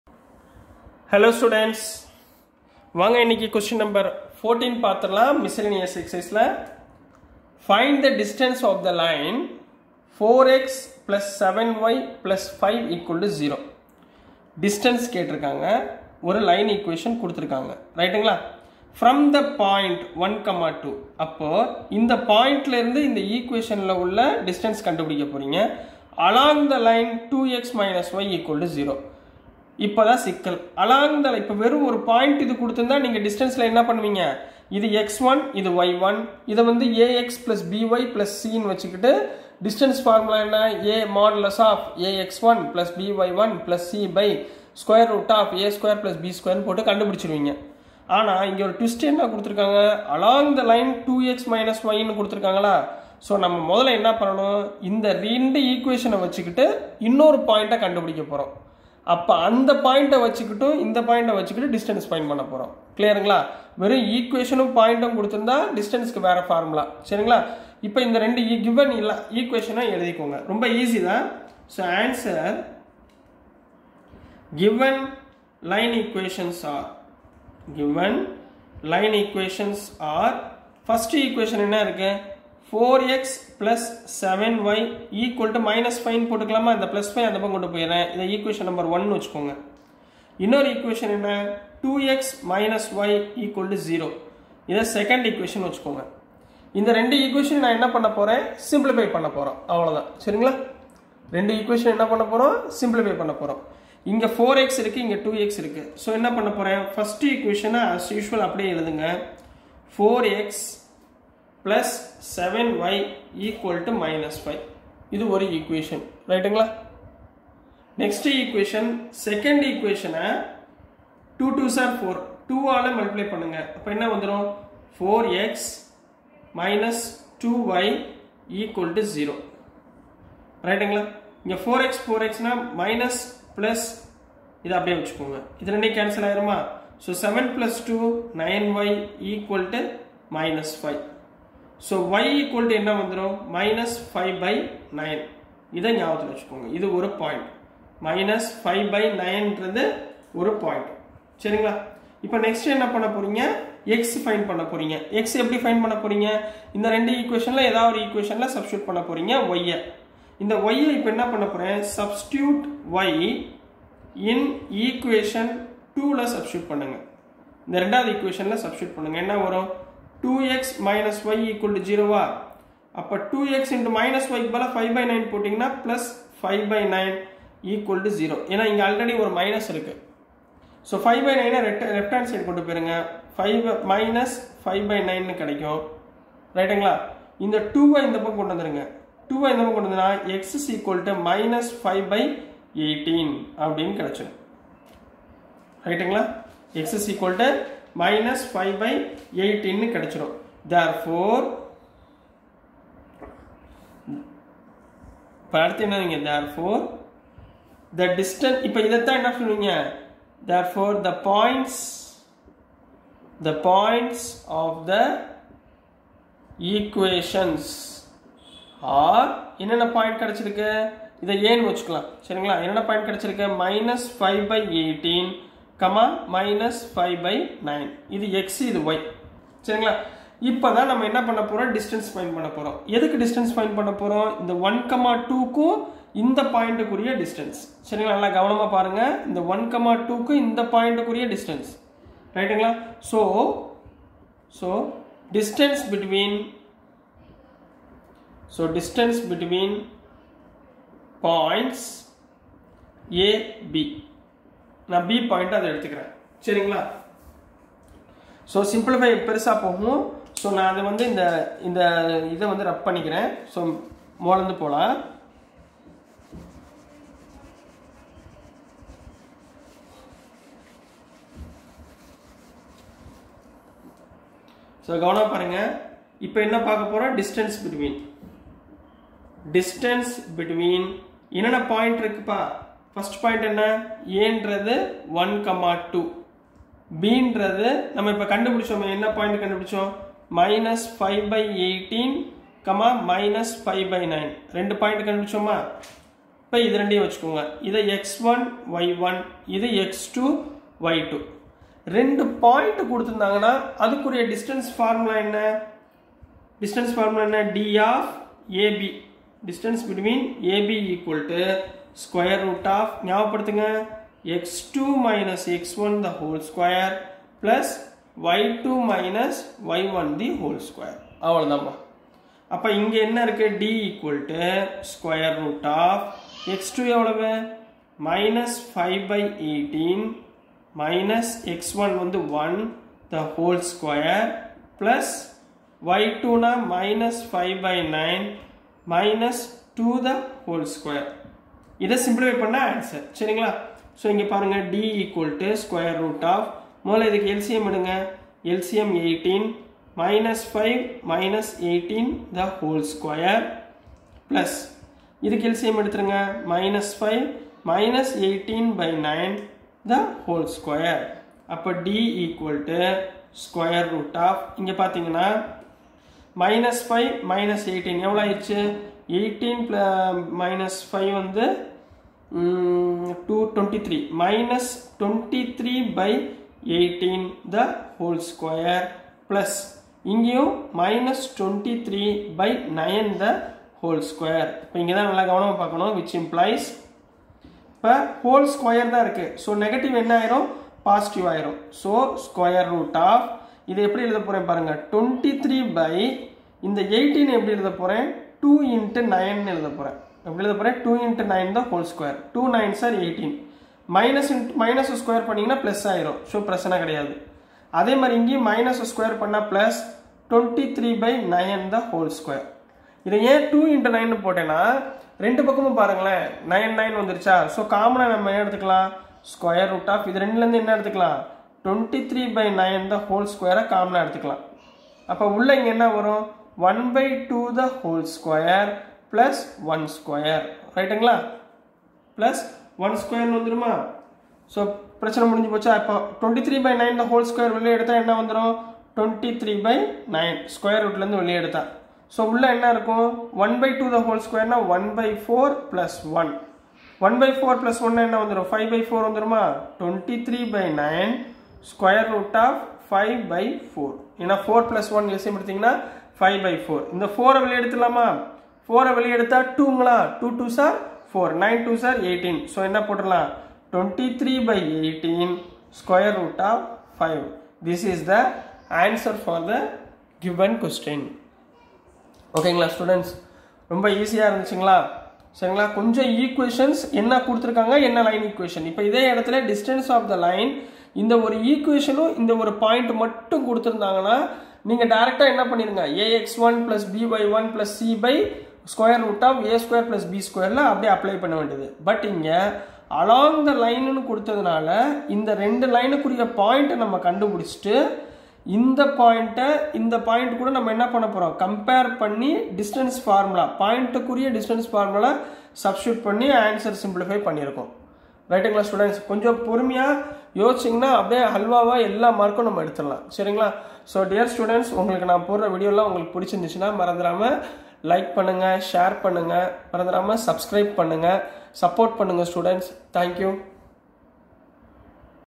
CLASS 11 MATHS CHAPTER 9 STRAIGHT LINES - MISCELLANEOUS EXERCISE Q 14 CBSE NCERT NEW SYLLABUS.
Explained about CLASS 11 CBSE NCERT NEW SYLLABUS MATHS CHAPTER 9 STRAIGHT LINES - MISCELLANEOUS EXERCISE PROBLEMS in tamil language.